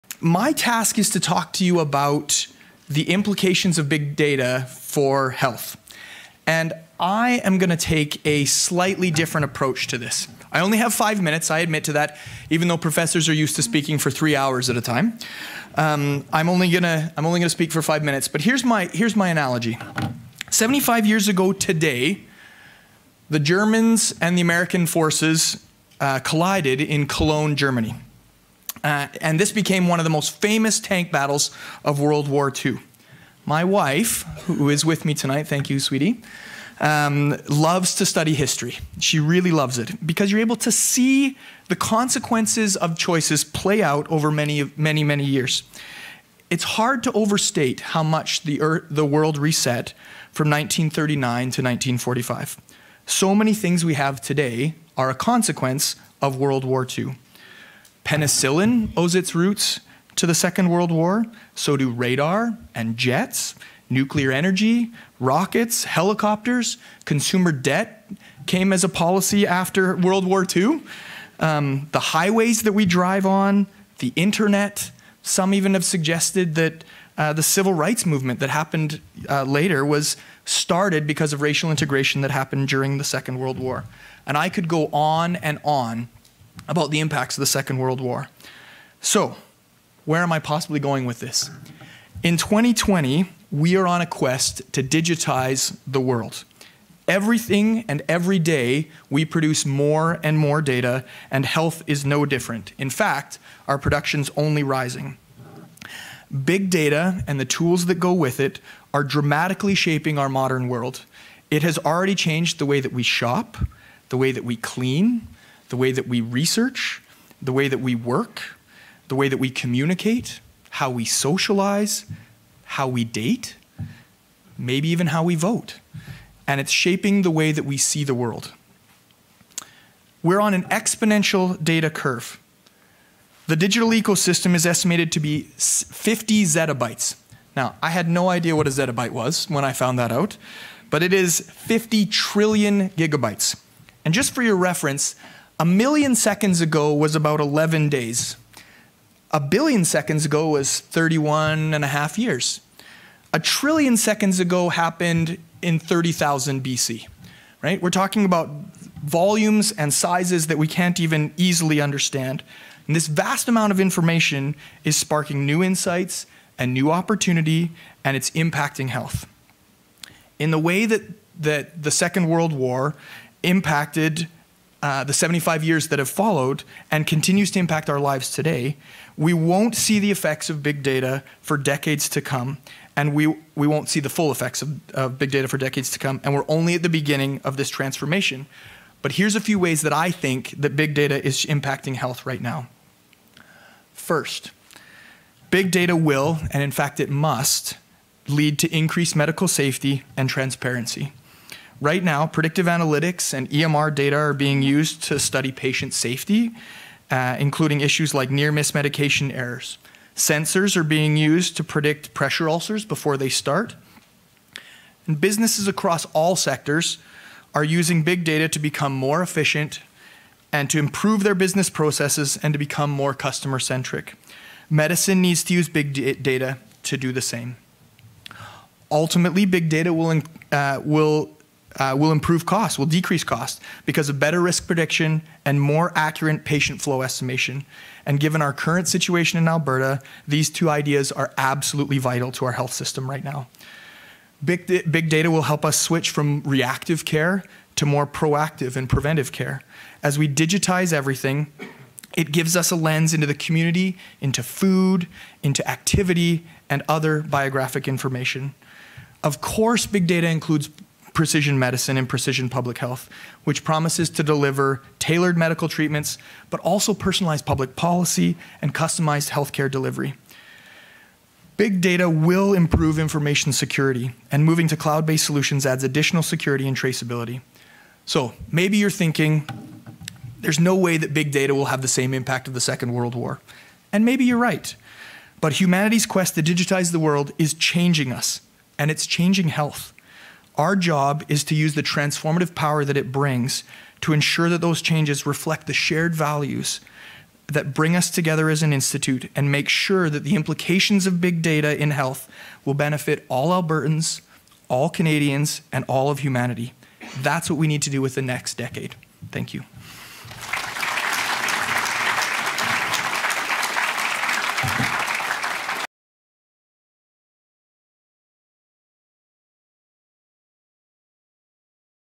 A new decade in public health: Implications of Big Data for Health
Dr. Tyler Williamson presented on 'Implications of Big Data for Health' at the O'Brien Institute for Public Health 10-years of IMPACT celebration: A new decade in public health.